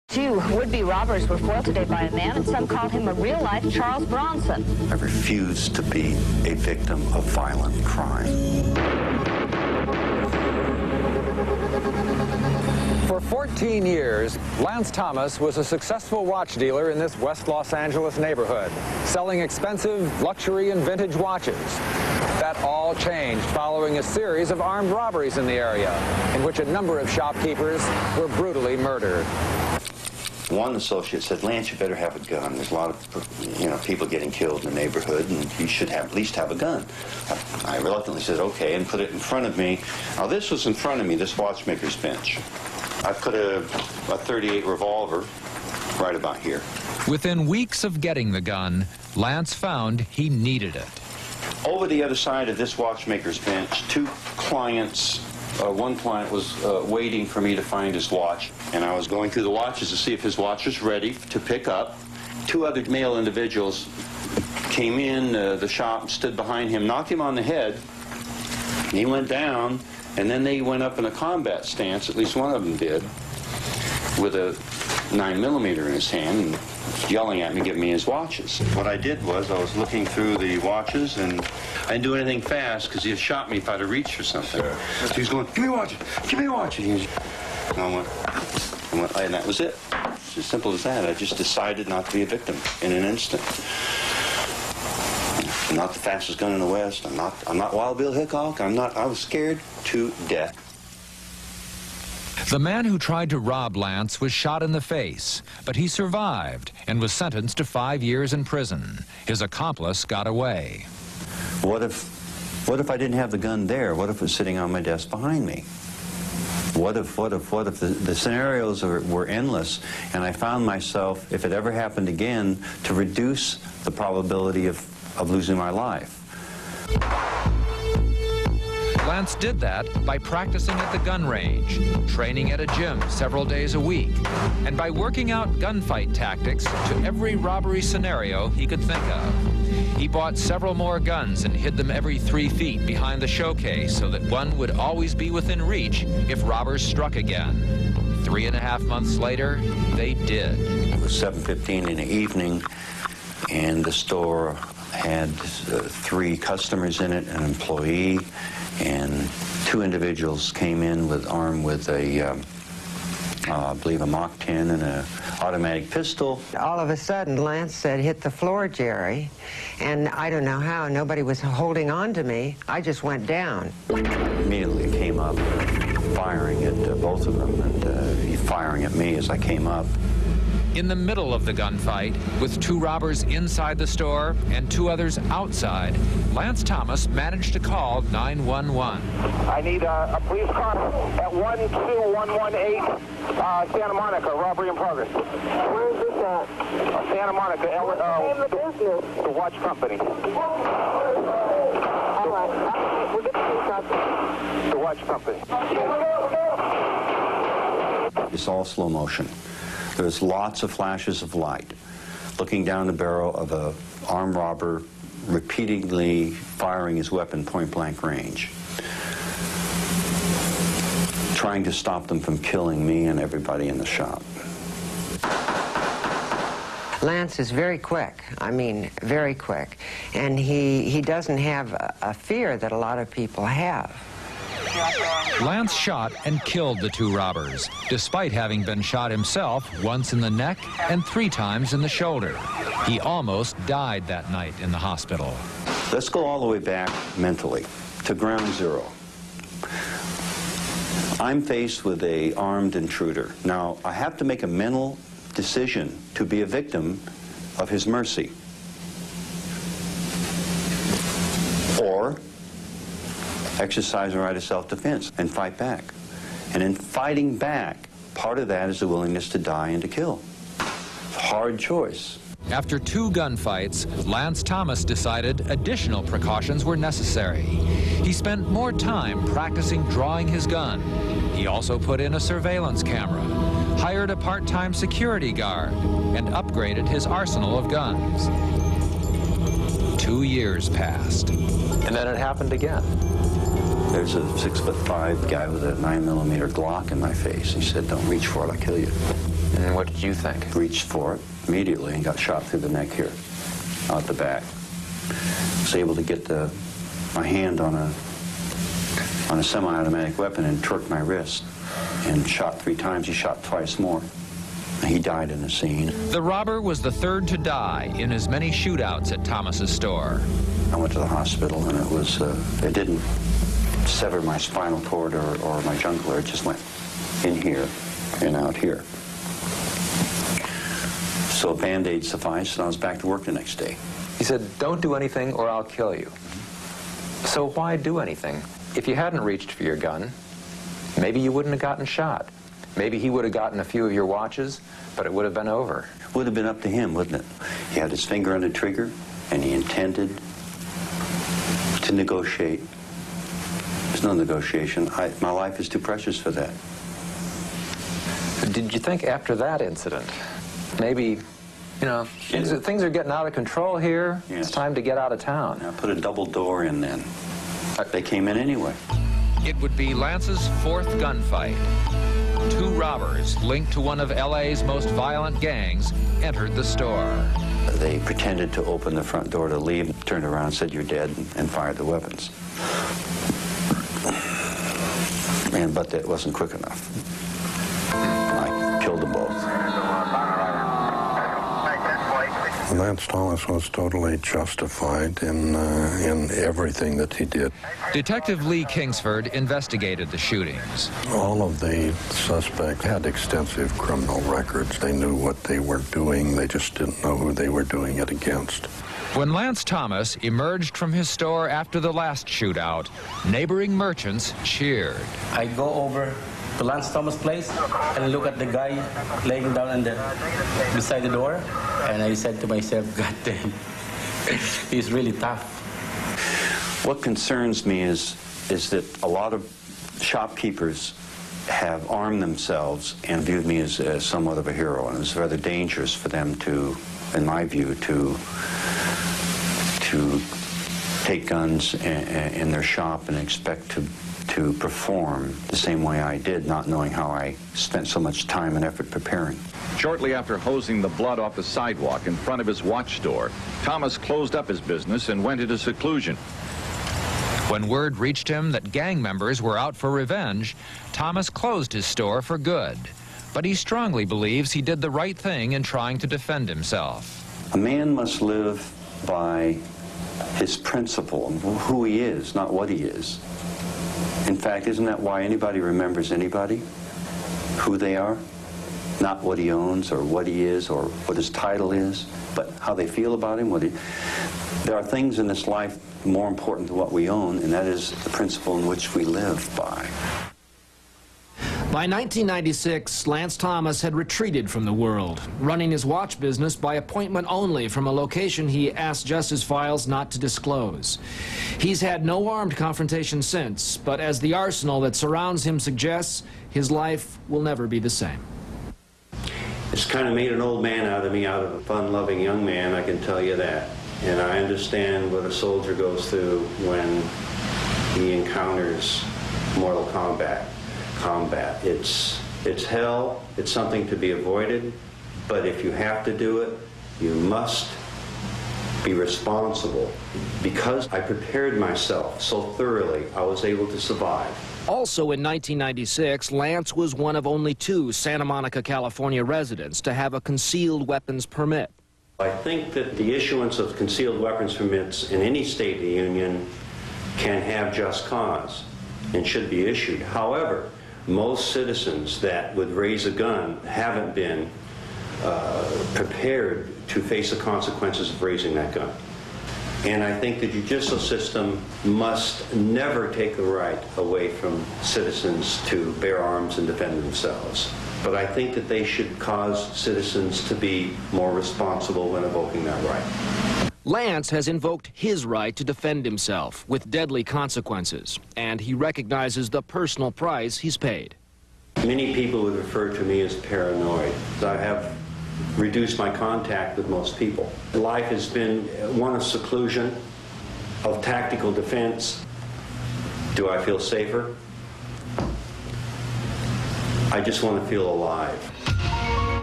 Lance Thomas - Fights Back At Robbers, And Wins
Lance Thomas ran a watch shop in Los Angeles for 14 years. He was robbed at gunpoint at least 3 separate times. Each time Lance fought back, and won!